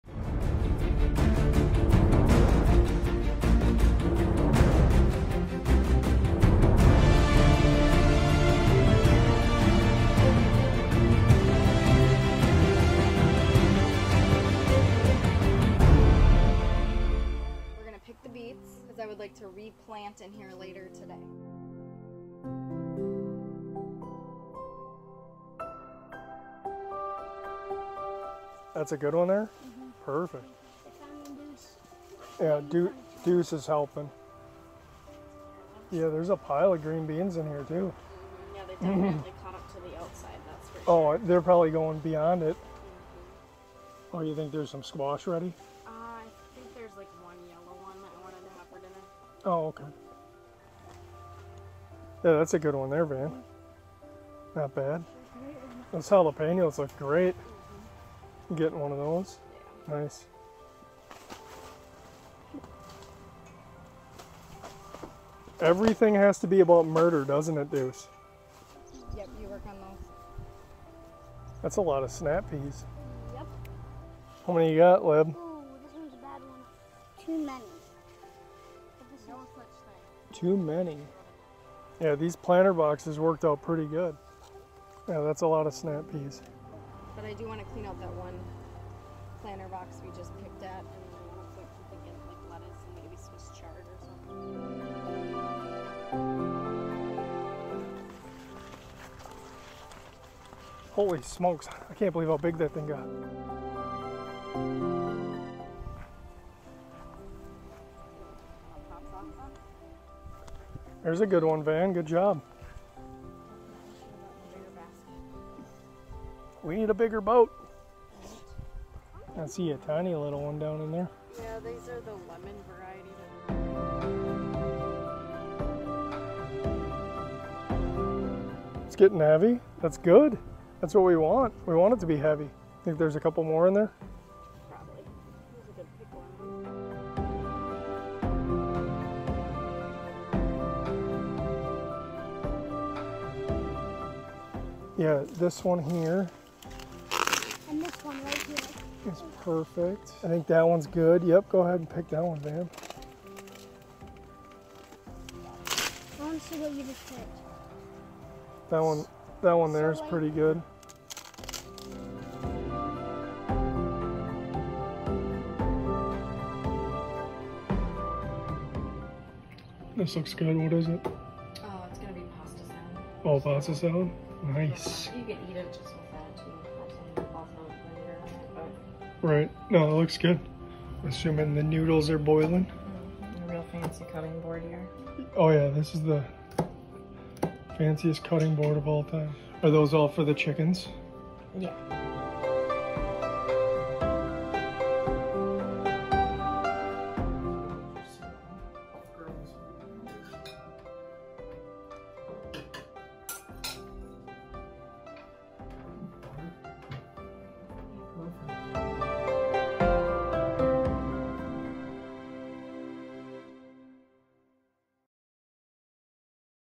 First BIG Garden Harvest || August 🌻 2022
In this video, We spent some time in the garden. Our first big vegetable harvesting video. This garden harvest net us 65 pounds of vegetables. Sweet Corn, Zucchini, Cucumbers, Beans, Peas, Tomatoes and Beets.

00:00 Welcome to Lohre Farm